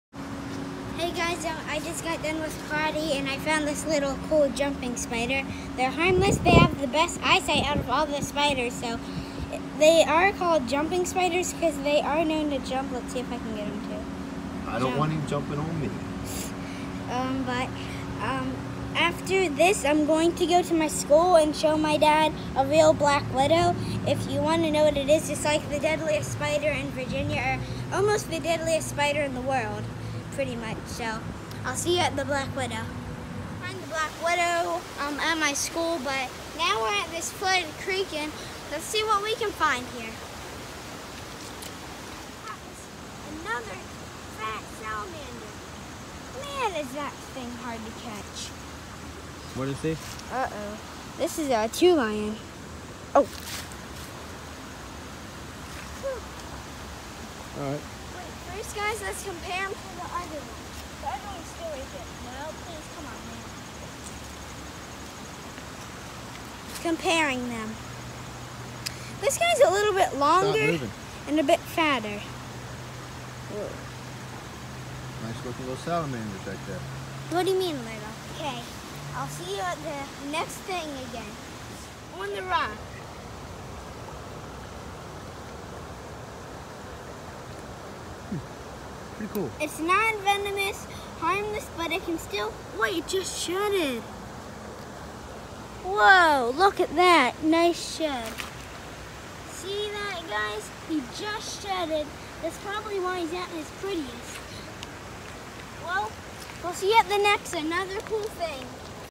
Spiders and salamanders
Finding some cool spiders and salamanders